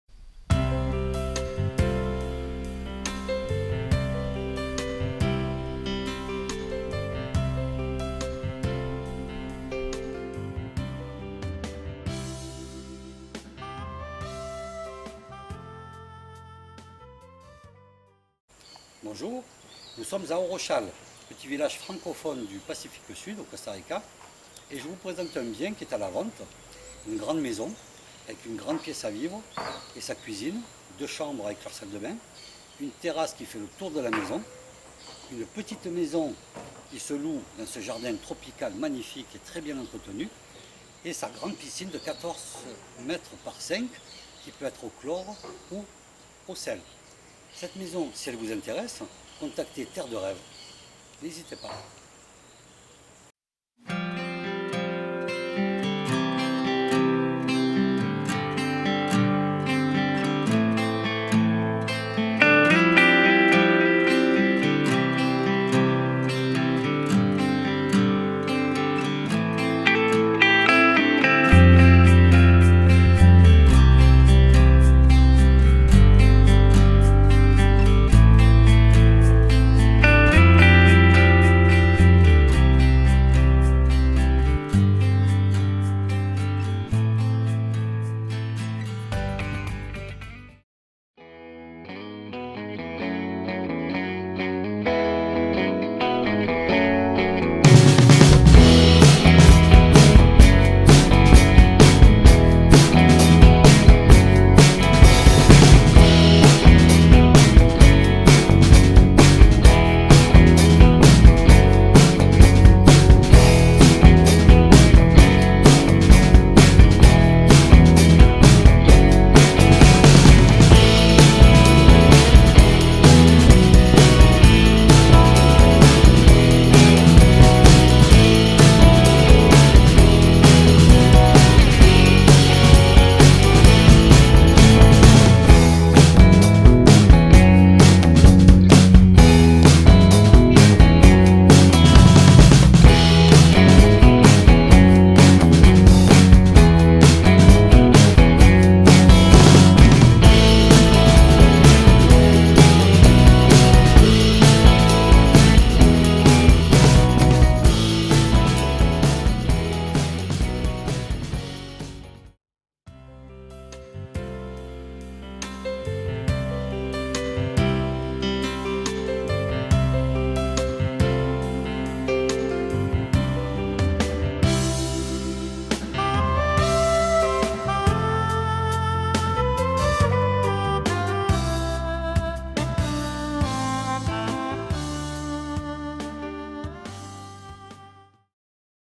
Très grande maison avec énorme piscine et maison d'hôtes à Ojochal Costa Rica
Gros potentiel pour cette maison et sa maison d'hôtes à Ojochal Costa Rica
Vendue meublée